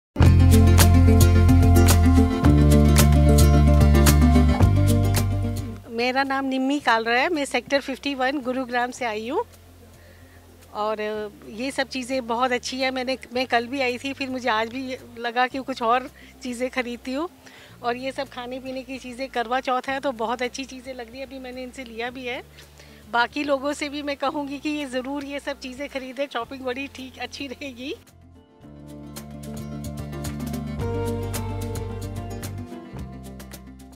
आगंतुक निम्मी कालरा जी ने सरस आजीविका मेला को हस्तनिर्मित उत्पादों की खरीदारी के लिए आदर्श मंच बताया
आगंतुक निम्मी कालरा जी ने बताया कि वो लगातार दूसरे दिन सरस आजीविका मेला, गुरुग्राम में खरीदारी के लिए आ रहीं हैं और वे बताती हैं कि सरस आजीविका मेला त्यौहारों और दैनिक आवश्यकता की वस्तुओं की खरीदारी के लिए एक आदर्श मंच है।